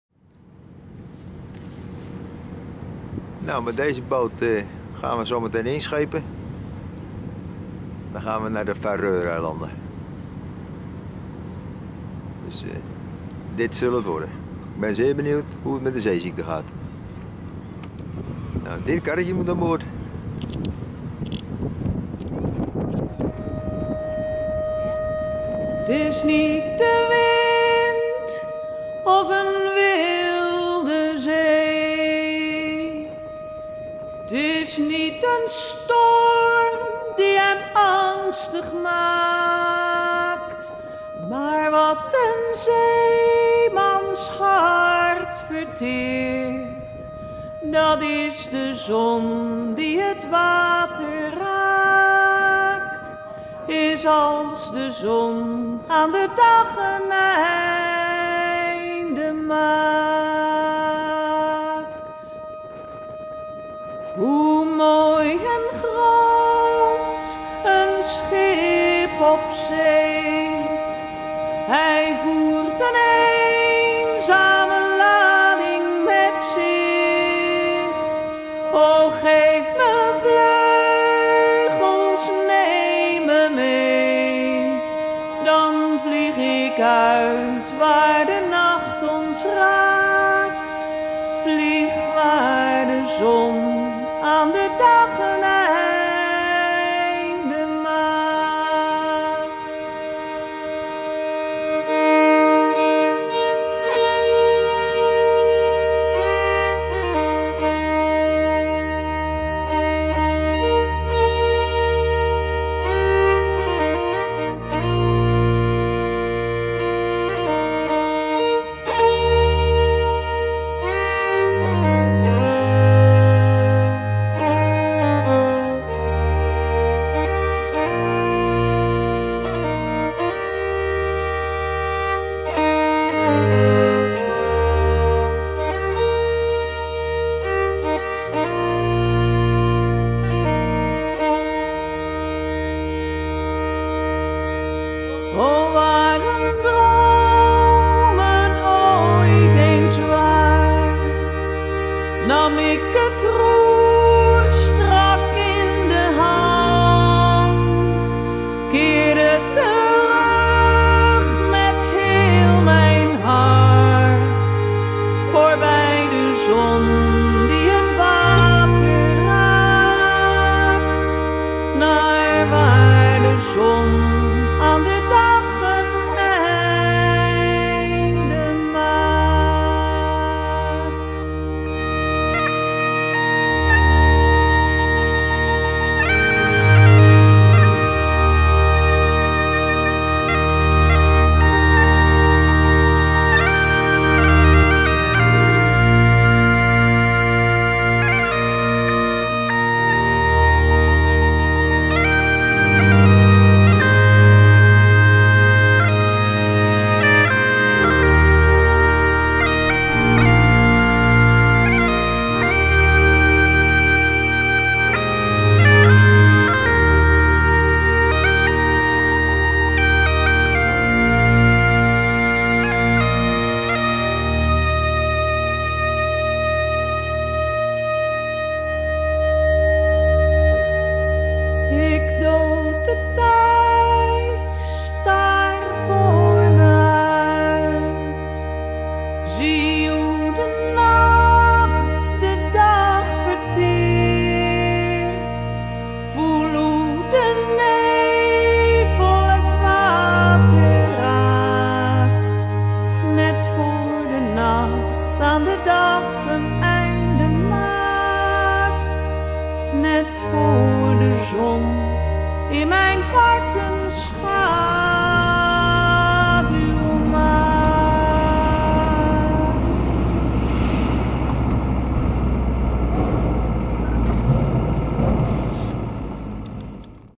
Hans & Margot op weg naar Faroer eilanden
Mooi gefilmd door Hans Weel en nu met goed bijpassende muziek gemonteerd. Let op de details.